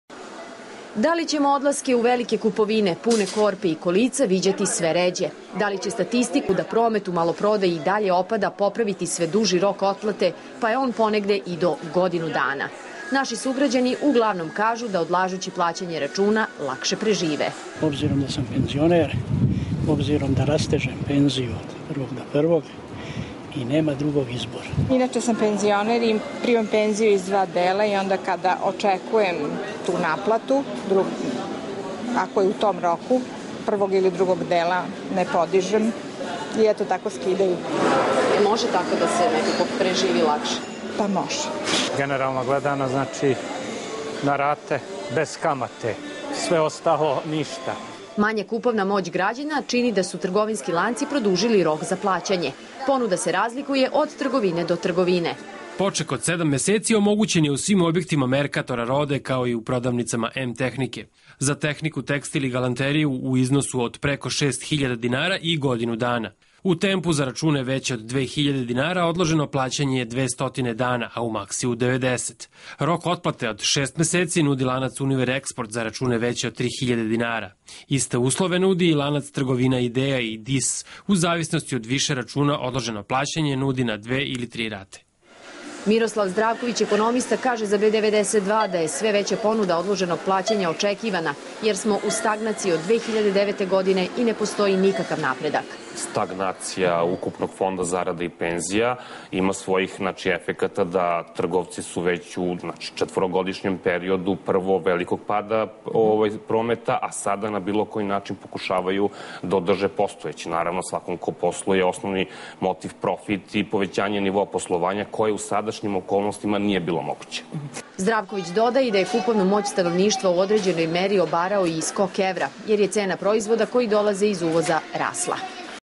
Život na odloženo
U Srbiji se kiselo našale da, kako kažu, žive na odloženo. Standard gradjana,
prema procenama ekonomista, u protekle četiri godine opao je za 60 odsto. Da
bi nekako prodali robu, trgovci u nekim trgovinskim lancima nude odloženo
plaćanje i do godinu dana.